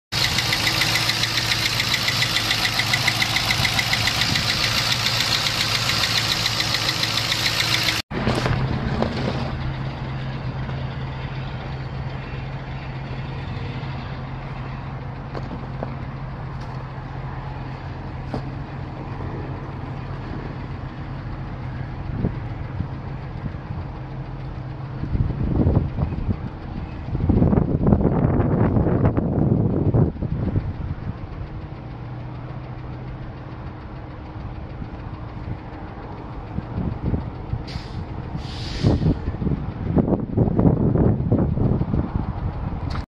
2014 Dodge Charger Police for sale at auction | bidding closes January 25, 2022
2014 Dodge Charger Police
 Miles: 151,627 on odometer
 VIN: 2C3CDXKTXEH279868
 Unit #: 1164
Engine
 Displacement: 5.7L V8 OHV 16V
 Cylinders: 8
 Fuel type: Gas
Transmission
 Automatic
 All wheel drive
Interior
 AC, Heat
 Cruise control
Tires
 Size: 225/60R18
Notes
 Windshield chipped or cracked
 Dead battery